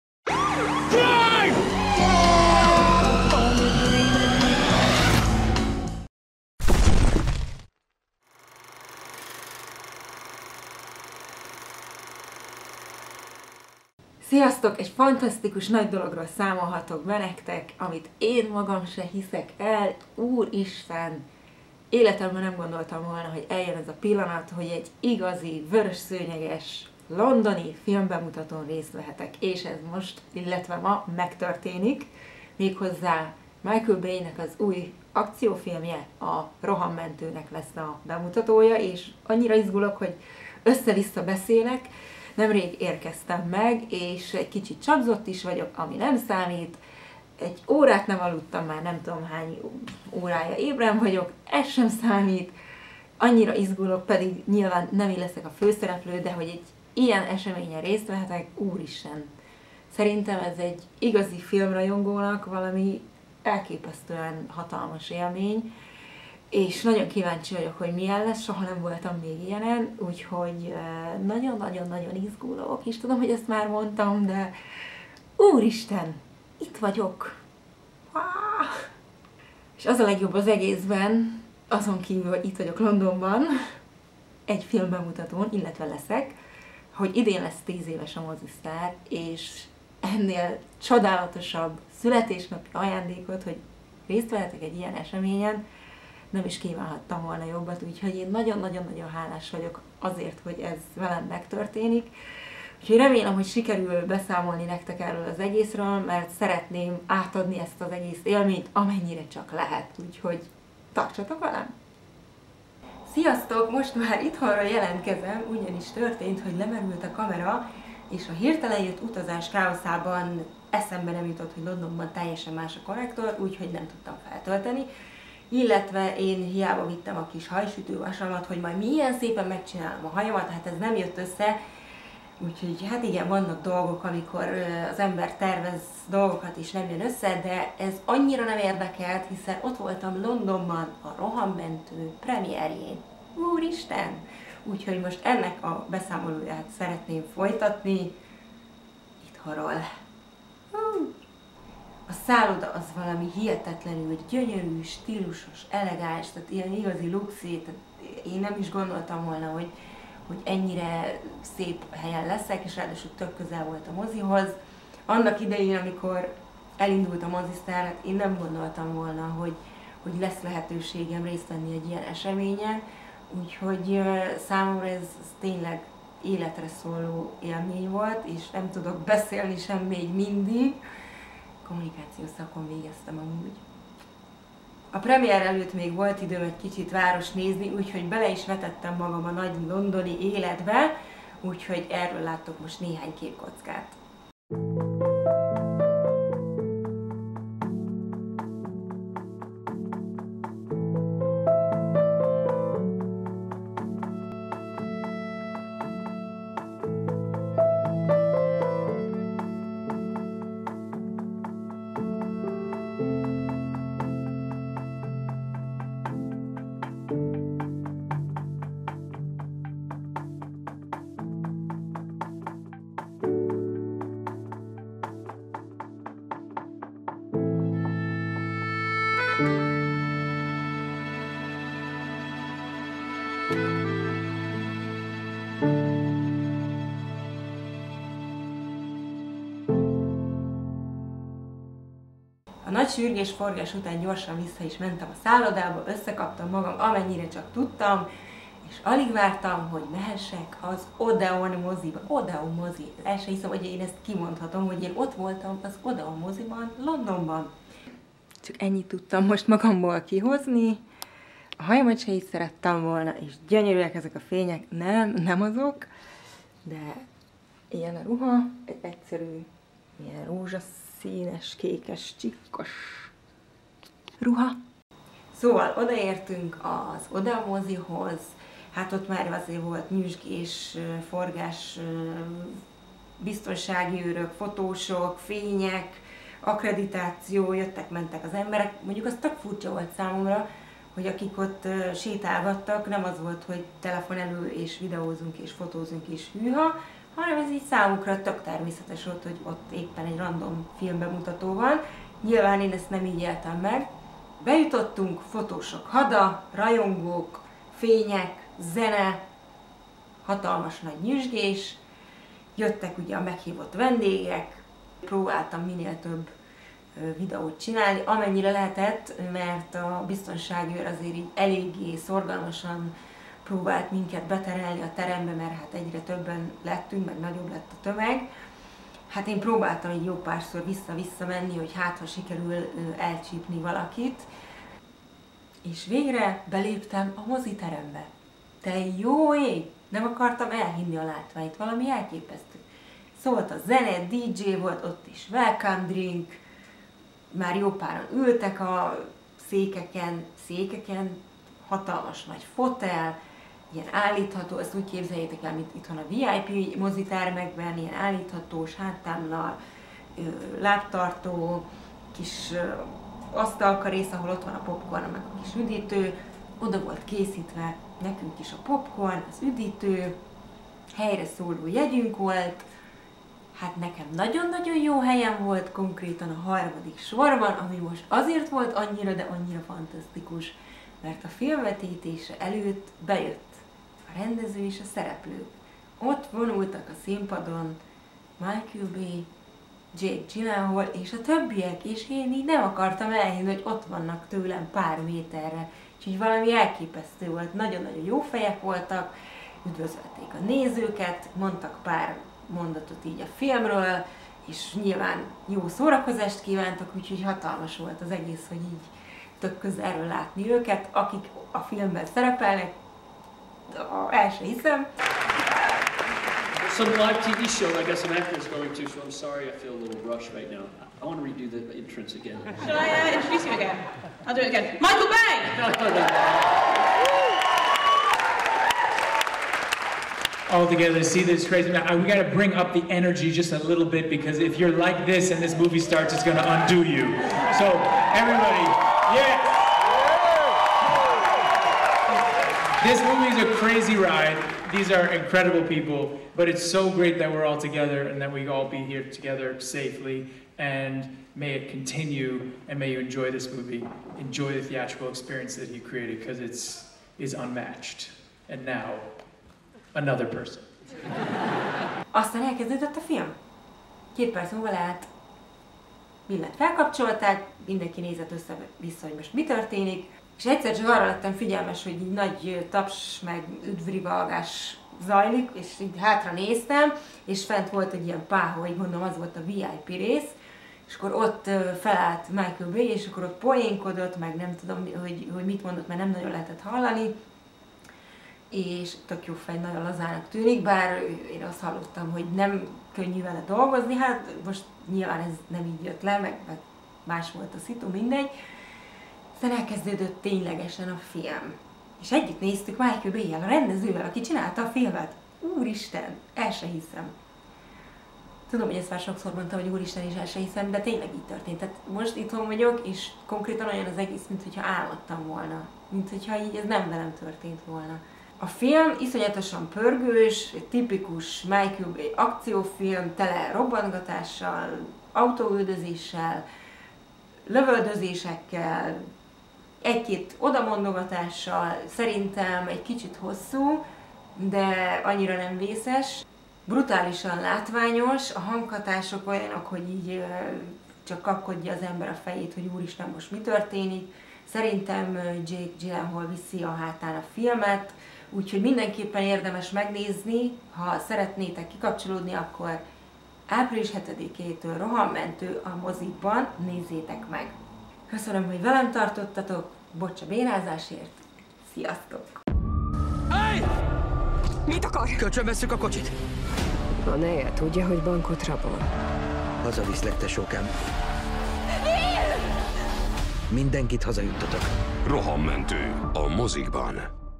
ROHAMMENTŐ - Londoni premier (Élménybeszámoló)
Április 7-én kerül a hazai mozikba a ROHAMMENTŐ című film. Íme egy kis videós beszámoló a londoni premierről, ahol részt vett a rendező, Michael Bay és a film főszereplői is: Jake Gyllenhaal, Eiza González és Yahya Abdul-Mateen II is.